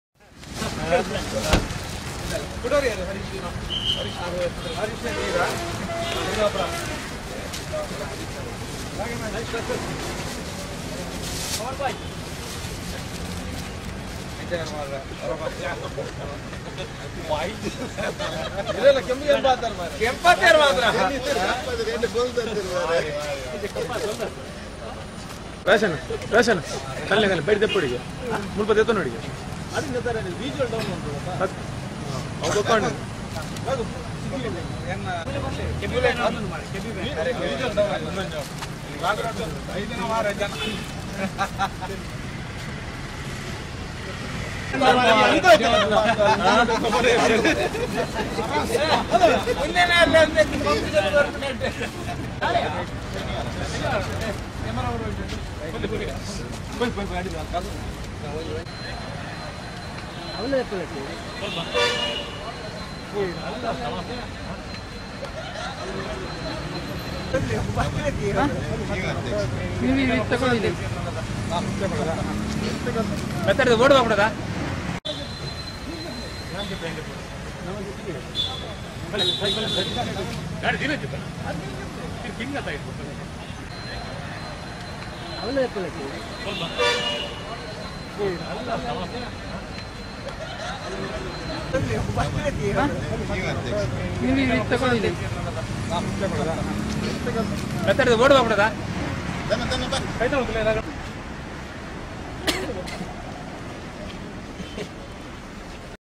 Udupi Photographers stranded in Kashmir Flood return home safely.
Udupi, Sep 13: Two photographers from Udupi who had been to Kargil on August 31 to take the pictures witnessed nightmare as they got stuck in heavy rain and flood in Kashmir.